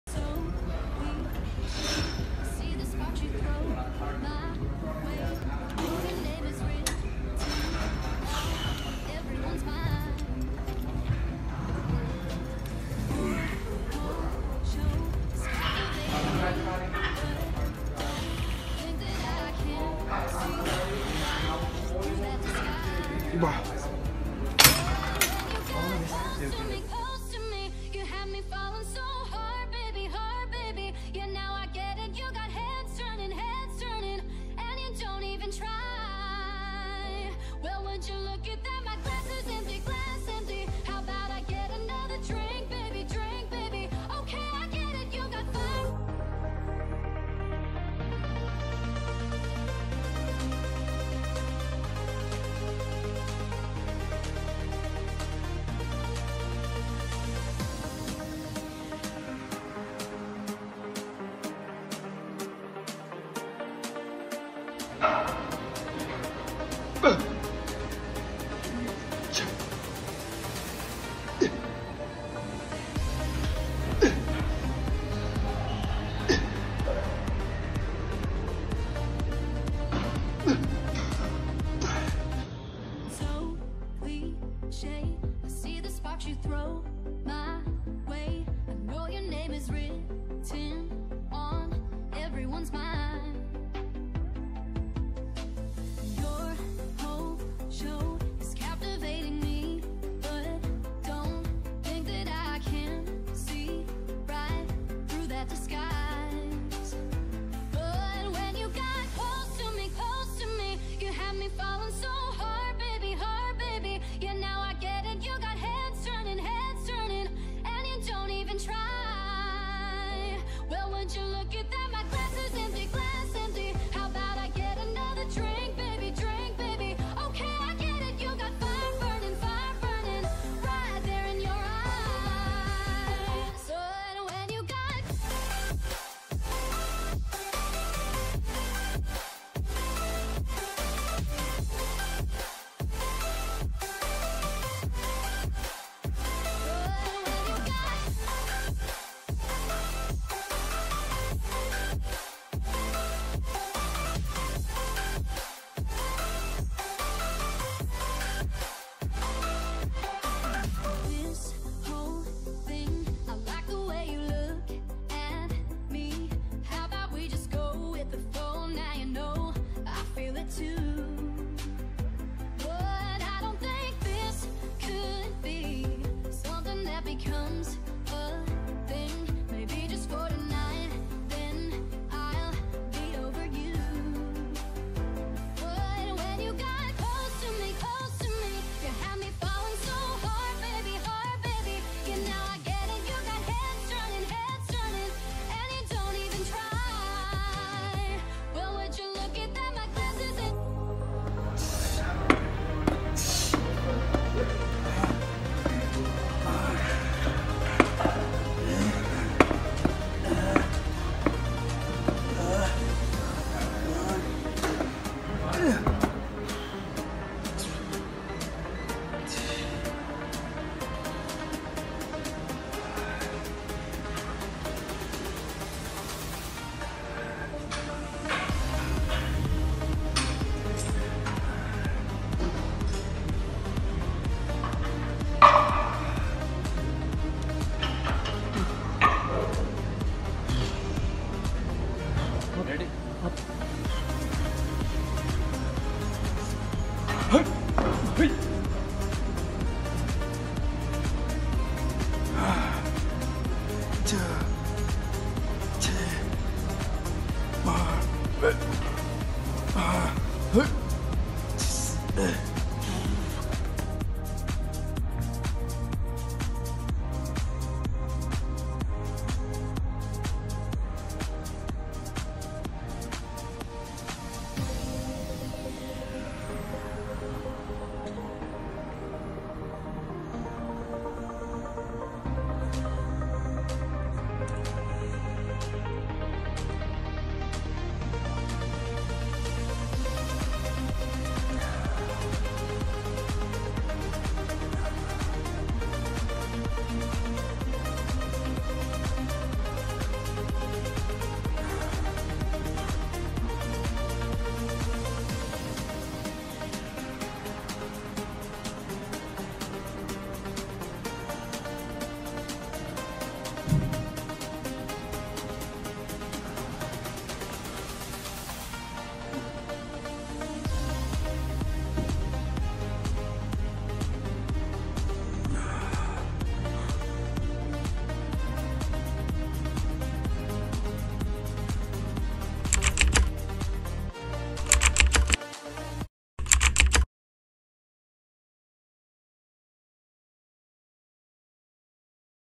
INTENSE UPPER BODY WORKOUT || SIX PACKS ABS || BICEPS || TRICEPS || CHEST WORKOUT || USANEPALI
Hy everyone I am back with another workout video.wanted to thank brother Rajan Rai for providing me this workout video.I hope this workout video gonna motivate every single person watching this video and hope will like it and love it.